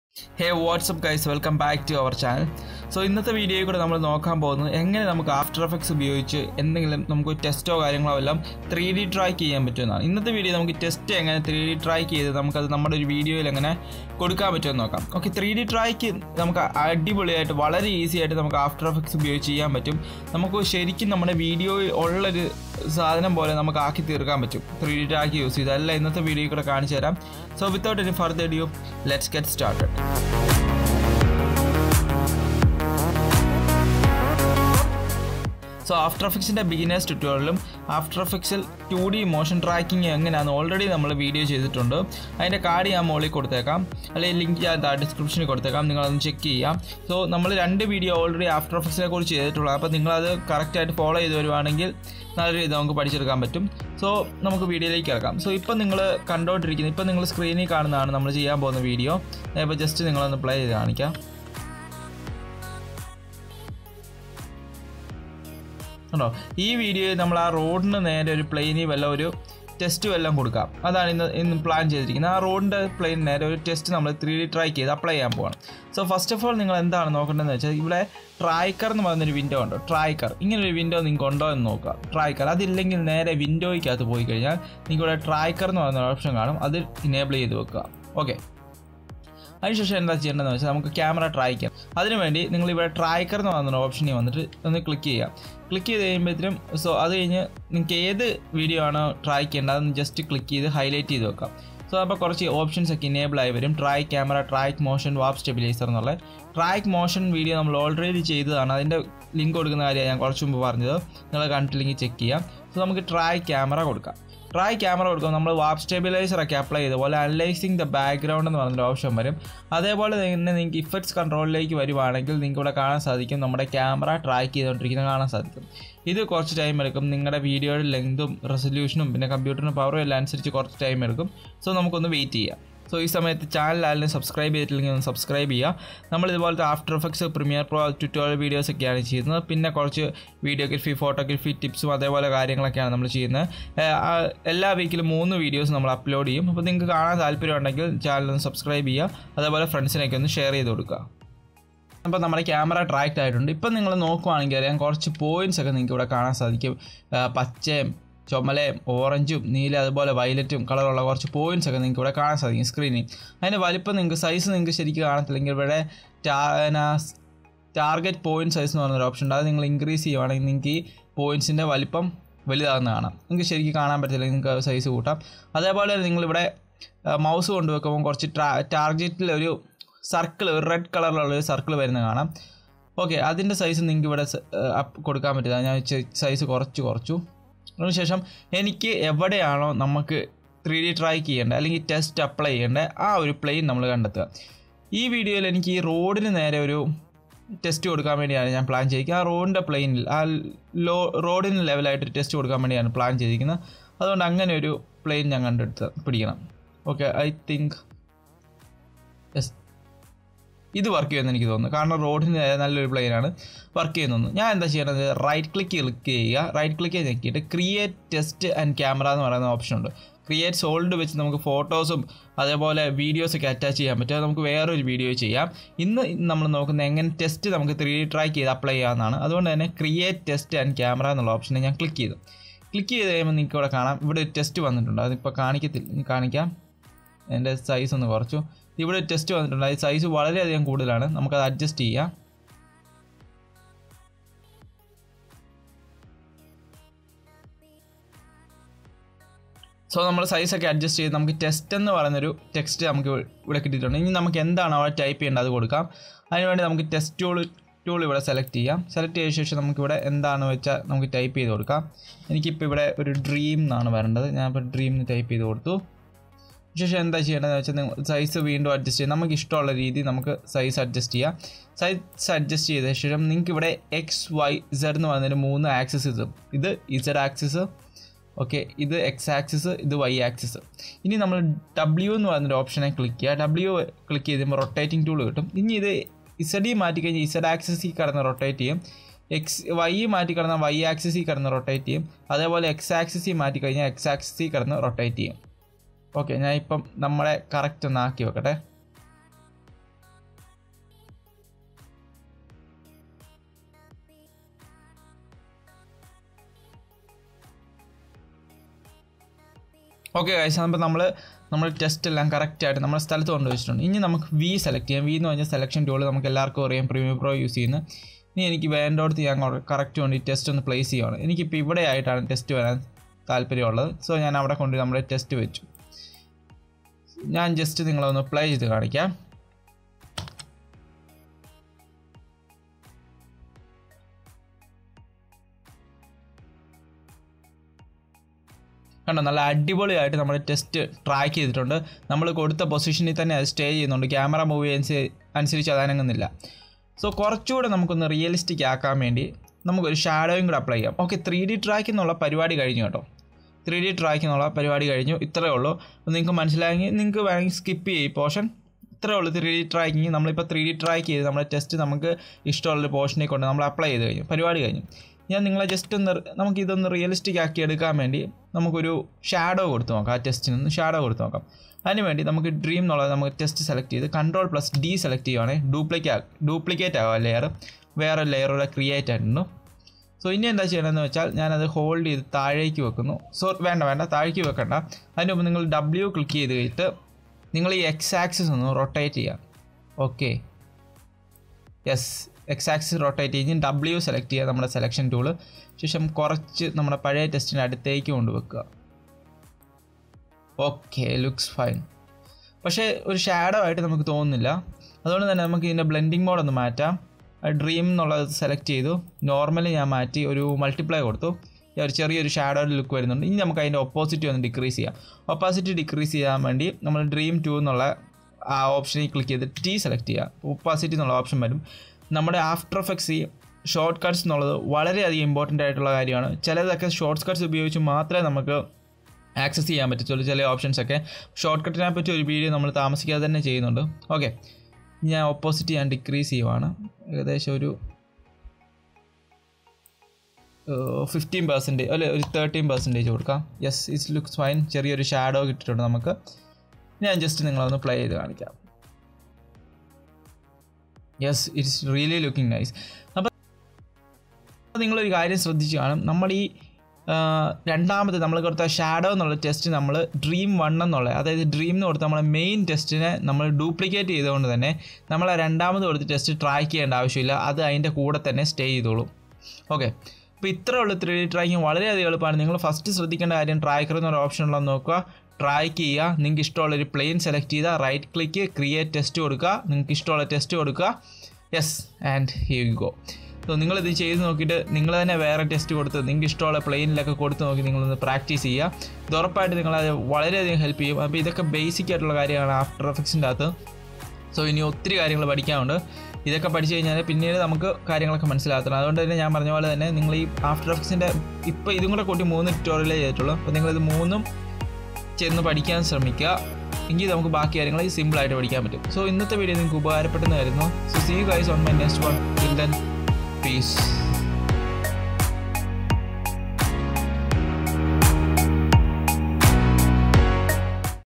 Learn 3D track (text) Camera In Aftereffects | Malayalam Tutorial
. mobile used       : lenovo a6020
. Video edited on : adobe premiere pro cc 2019
. Audio edited on :adobe audition cc 2019
Music and sound effects (credits)
Free images and videos (credits)